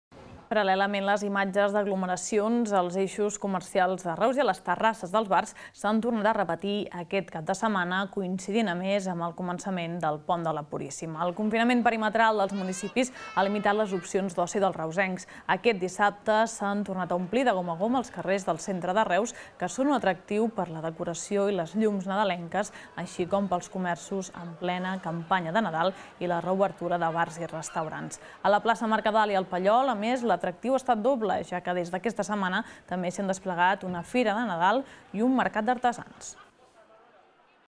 Els carrers de Reus repeteixen aglomeracions
Segon cap de setmana des de l'arrencada de la campanya de Nadal a Reus i els carrers del centre tornen a omplir-se de gent.